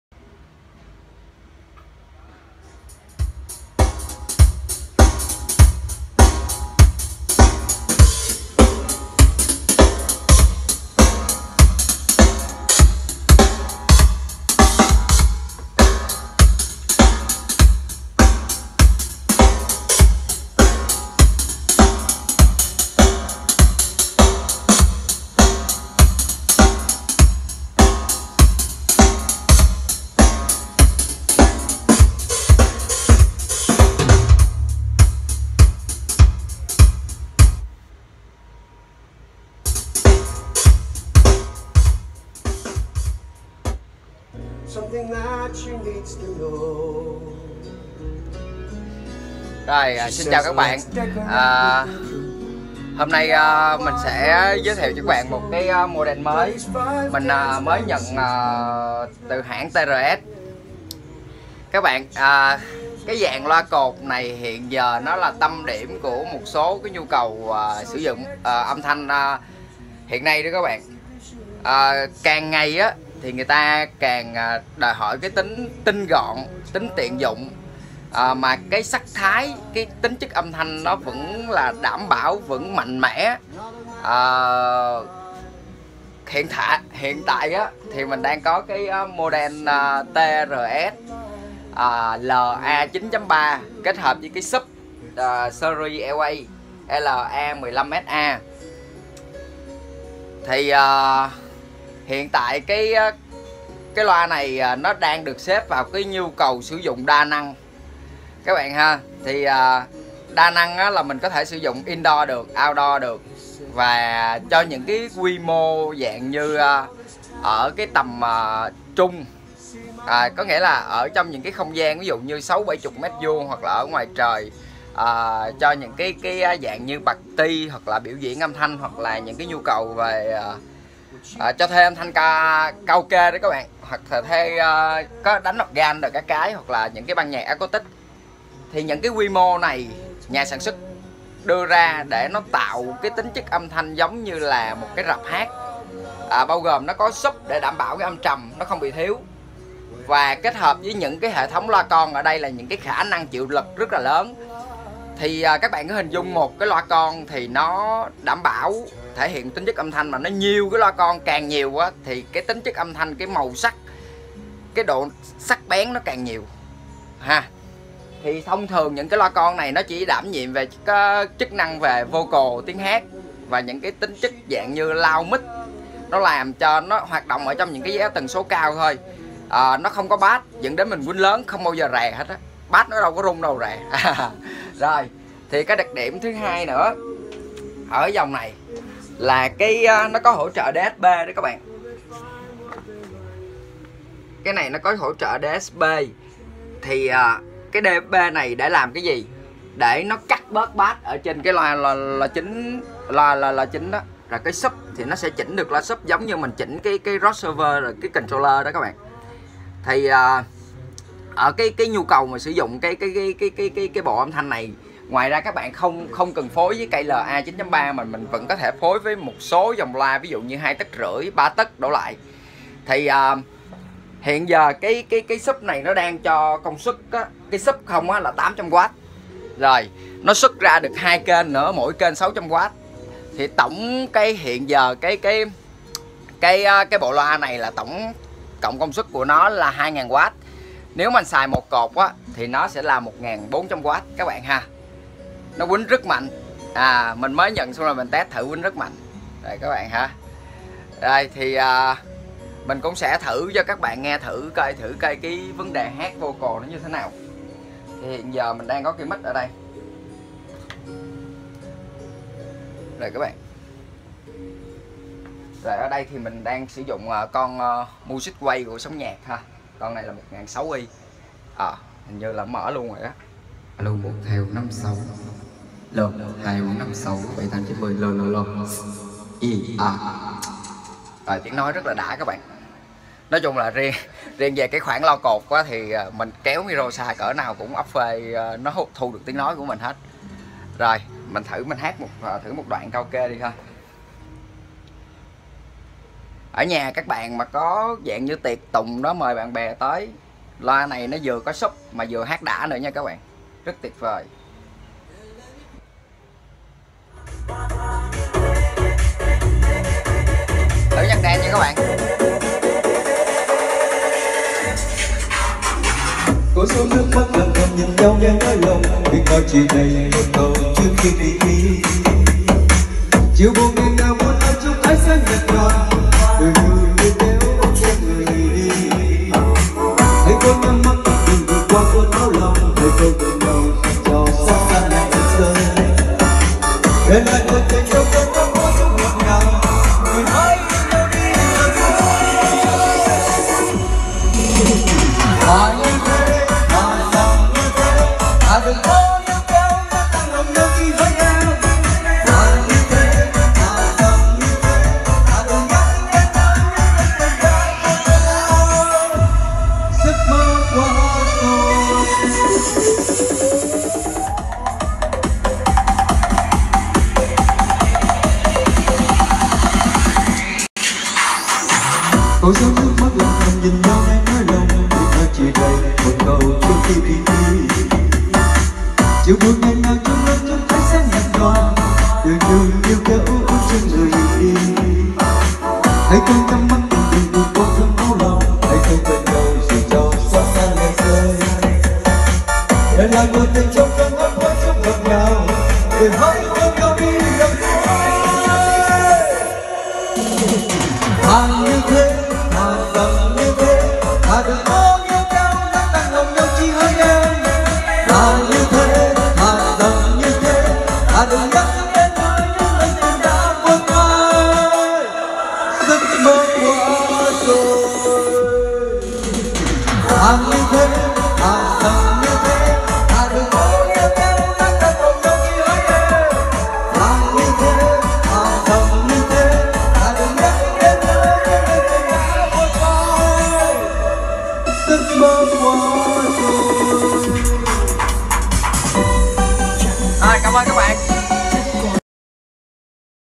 DEMO Loa cột PA TRS LA-15SA / LA-9.3, hàng mới về 2019
Thừa hưởng từ Series LA chuyên nghiệp, TRS cho ra đời Model TRS LA-15SA Subwoofer hổ trợ DSP (digital control), kết hợp cột loa LA-9.3, mang đến cho người dùng âm sắc phong phú từ nhà sản xuất loa hàng đầu thế giới, khảng định một chất lượng ấn tượng.

TRS LA-15SA/LA-9.3 là tầm nhìn từ nhà thiết kết TRS, sử dụng đa năng, nhỏ gọn và rất linh hoạt tăng, tăng cường âm thanh cho những nơi vừa và nhỏ, với yêu cầu về chất lượng âm thanh như nhà hát.

Nhà hàng tiệc cưới, hội trường, outdoor party, DJ Party và biểu diễn live band hoàn toàn phù hợp với khả năng trình diễn của loa

LA-9.3 sự kết hợp 9 loa 3 inch công nghệ từ neodyum

 Thông số kỹ thuật:

LOA CỘT LA-9.3

Đơn vị: 3''x 9 Đơn vị: 3''x 9 LA-9.3 sự kết hợp 9 loa 3 inch

Tần số thấp nhất: 90Hz

Cao nhất đạt 18Khz

Khả năng đáp ứng 94dB Peak: 135dB

Trở kháng 4 ohms

Kích thước ( WxHxD ) : 100x787x122mm

LOA SUBWOOFER  LA-15SA

Đơn vị bass: 15 inch 220 từ 100 lõi

15 inch cực thấp

Tần số thấp nhất 35Hz

Khả năng đáp ứng 98dB

Công suất 800W + 600W x 600W

Trở kháng 8 ohms

Đáp ứng tần số 30-300Hz

Kích thước ( WxHxD ) : 435x590x522mm

TRS Chính hãng